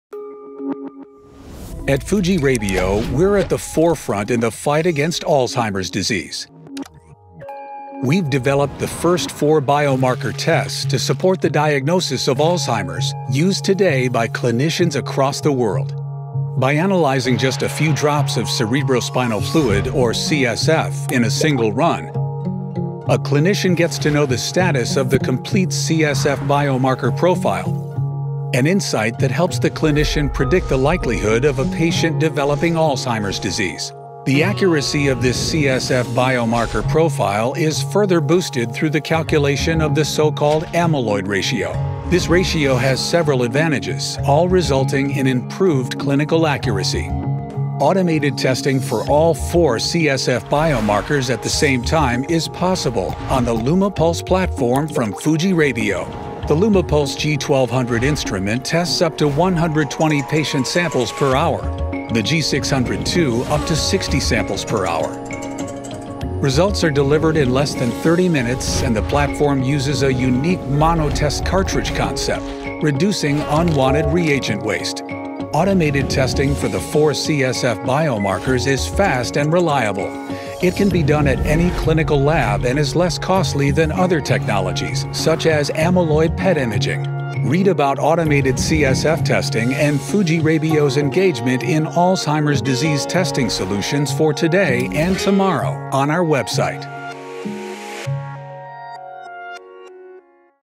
What is the interest of automated testing for all four main CSF biormarkers for Alzheimer's disease?
Automated testing for the four main cerebrospinal fluid (CSF) biomarkers to help with early and reliable Alzheimer's disease diagnosis is possible today.

In less than 2 minutes, this video explains the advantages of automated biomarker testing for all four CSF biomarkers.